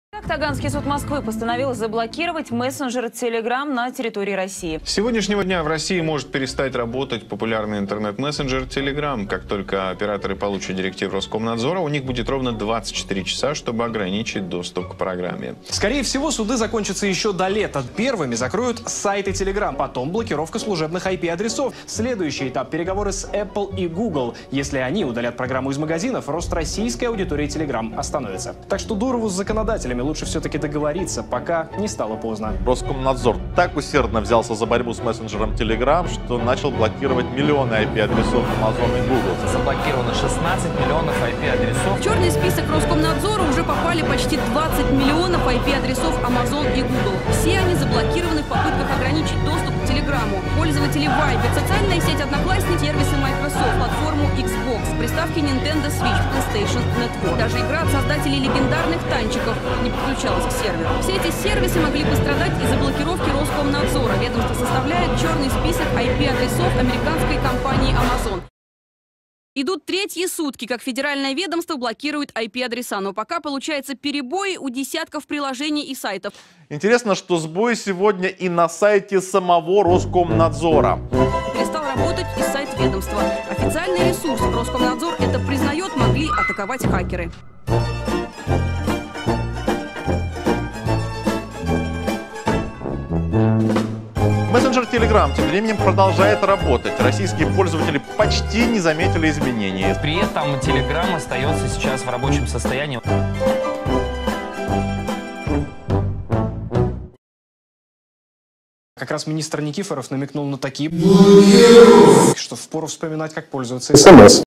Curb your blockages (Telegram vs Roskomnadzor) meme
Wasn't sure if i should make this meme after 2 weeks or not but as far as Telegram is still working fine i did it.
Video is on russian language  but i translated it to english with subtitles. I hope my London are gut.
A short story of what happened half a month ago on the russian internet. Roskomnadzor(federal department) decided to block Telegram messenger bcoz they didn't give encryption keys to Federal Security Service.Watch the video to see what happened next!